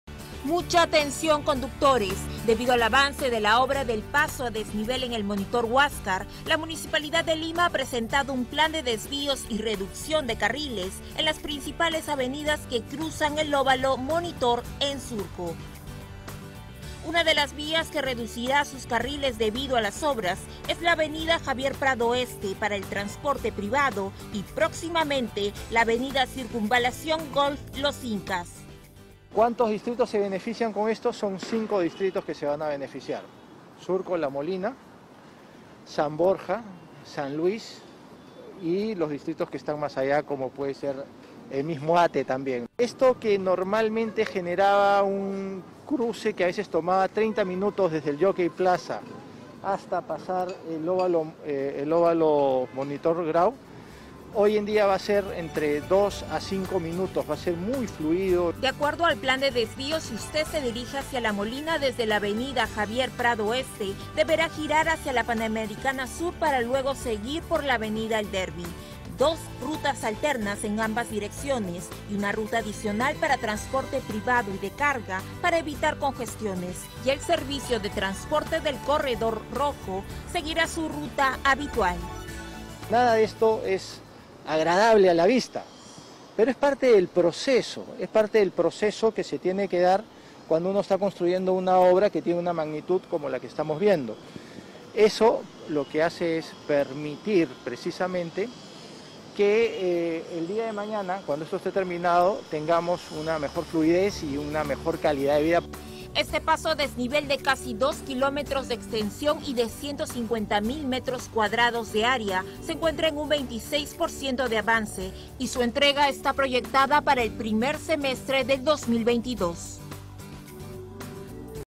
MML: Obras del paso a desnivel en óvalo Monitor Huáscar tienen 26 % de avance
Mucha atención conductores debido al avance de la obra del paso a desnivel en el Monitor Huáscar, la Municipalidad de Lima ha presentado un plan de desvíos y reducción de carriles en las principales avenidas que alimentan el óvalo Monitor en Surco.

Este paso a desnivel de casi 2 km de extensión y de 150 mil metros cuadrados de área, se encuentra en un 26% de avance y su entrega está proyectada para el primer semestre del 2022.

PLAN DE DESVÍO

De acuerdo al plan de desvío si usted se dirige a La Molina desde la avenida Javier Prado Este, deberá girar hacia la Panamericana Sur para luego seguir por la avenida El Derby.

Dos rutas alternas en ambas direcciones y una ruta adicional para transporte privado y de carga para evitar congestiones, y el servicio de transporte del corredor rojo seguirá su ruta habitual.

Una de las vías que reducirá sus carriles debido a las obras es la Av. Javier Prado Este para el transporte privado y próximamente la Av. Circunvalación Golf Los Incas.